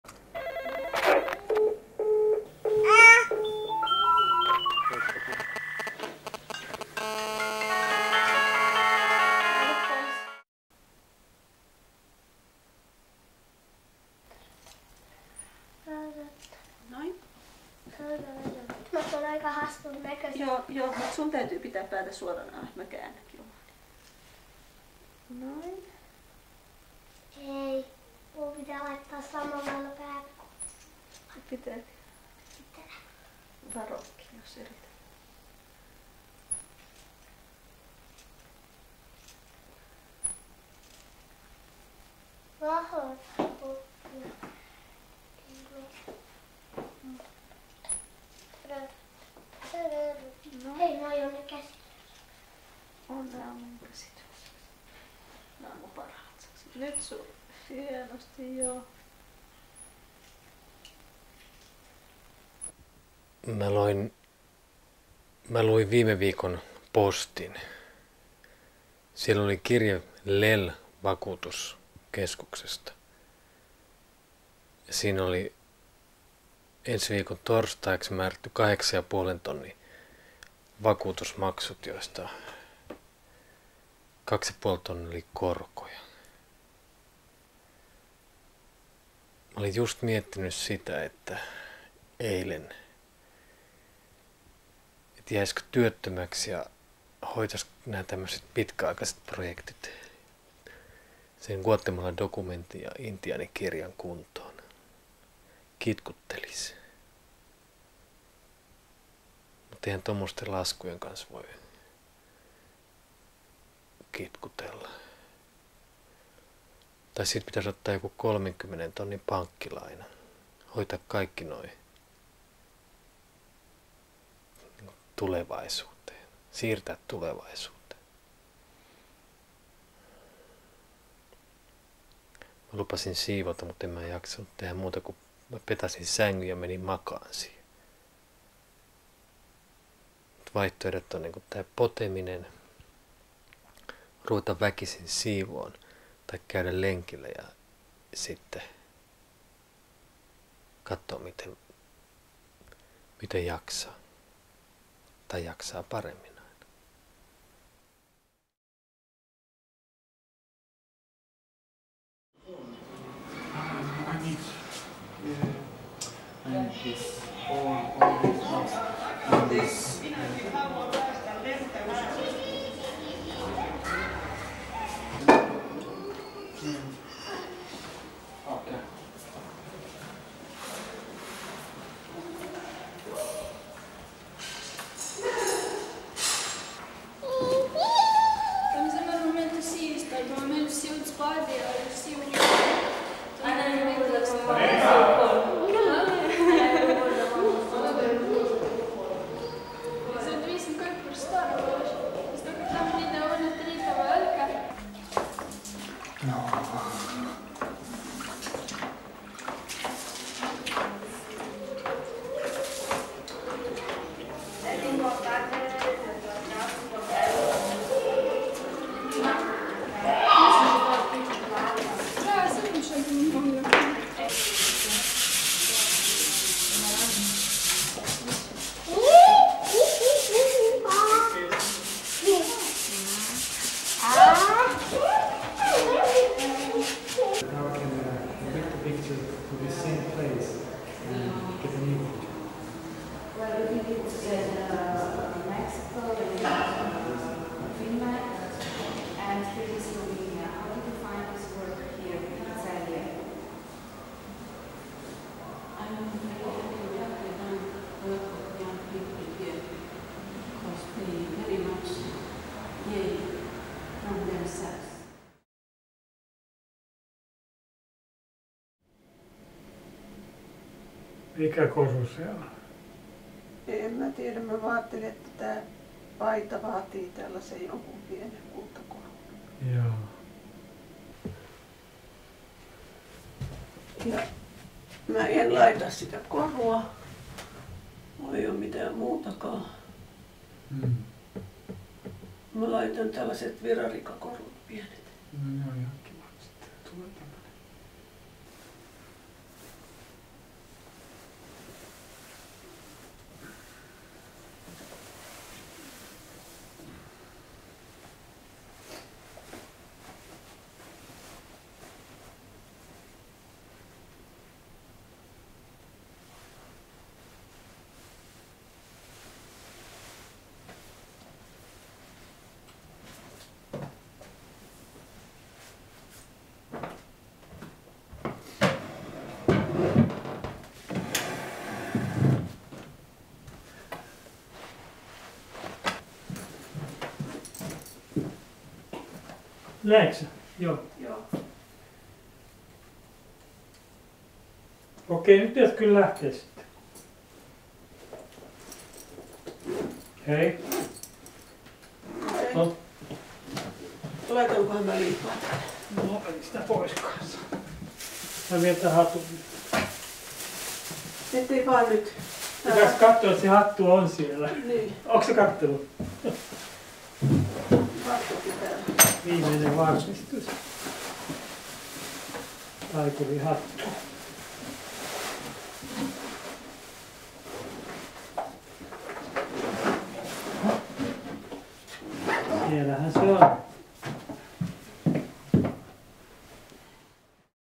Episode 1181: How to dress for the doctoral examination. June 5 / Year 3
Episode 1181: How to dress for the doctoral examination

From three decades

Pekka K is a Finnish video artist who has kept a video diary daily since March 1990.